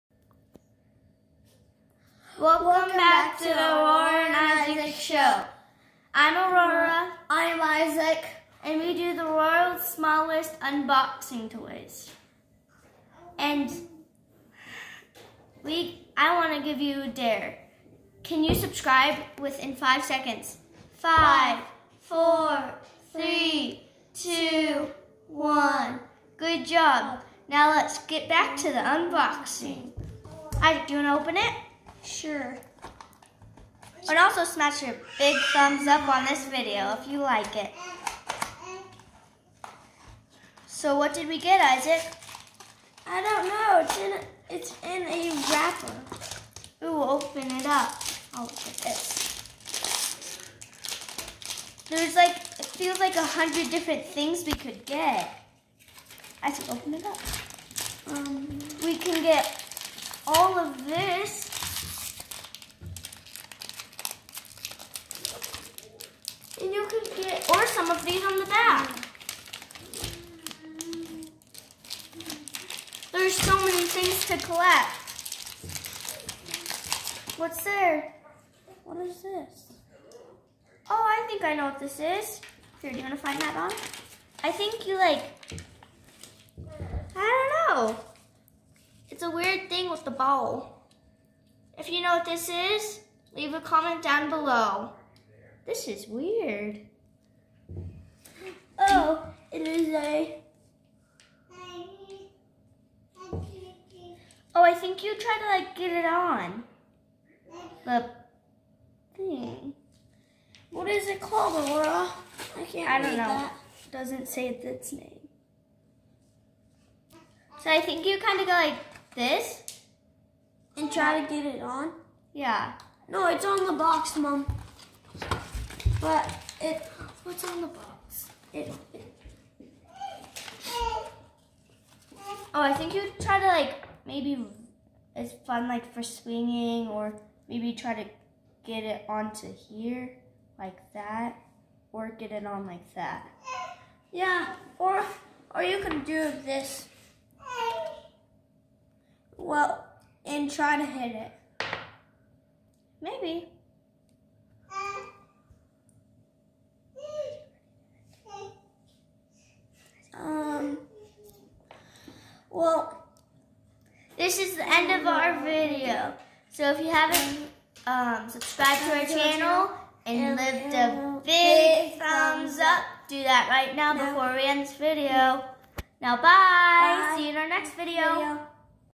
World's Smallest Toy Unboxing
The kids do another unboxing to see what worlds smallest toy they will get this time.